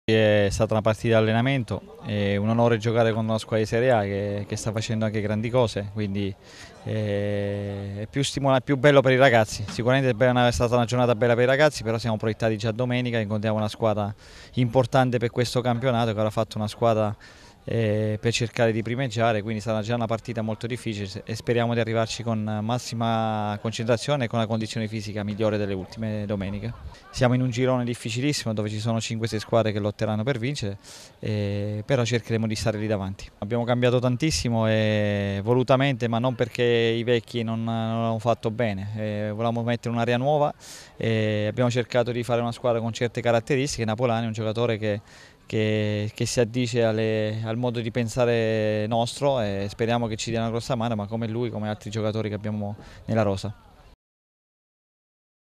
Epifani. Domenica parte il campionato del San Nicolo'
Domenica parte il campionato di Serie D. per il san Nicolò di Massimo Epifani si comincia con l'impegno interno contro la Fermana. Le impressioni del tecnico